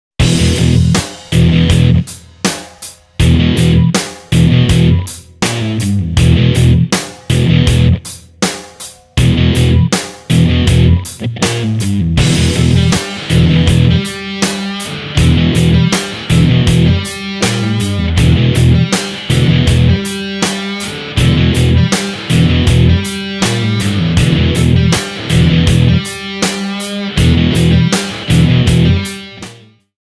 High-Leverage Diagonal-Cutting Pliers – Angled Head – Rebar Work
Popular with Ironworkers, these diagonal-cutting pliers were designed for rebar work. The 13° degree angled head makes it easier to work in confined spaces while the hooked handle improves the grip and reduces slippage.